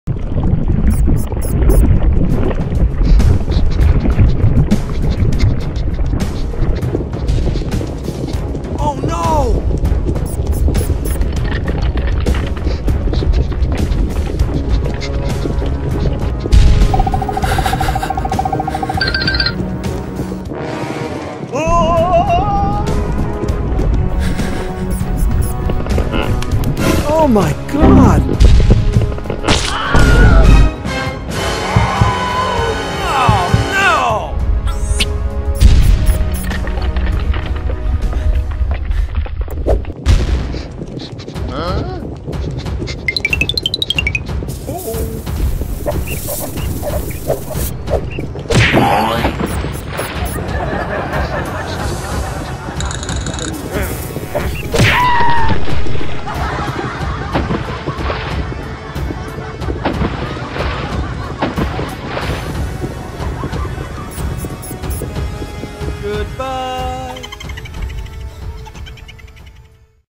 Hamster Overcomes Lava Floors of the Volcanic
Oh, our Hamster hamster unfortunately gets lost in the labyrinth trap Lava Floor of the Volcanic.
Let's see how she went to the end of the challenging journey.
Now, it's showtime!
Have you watched these videos yet?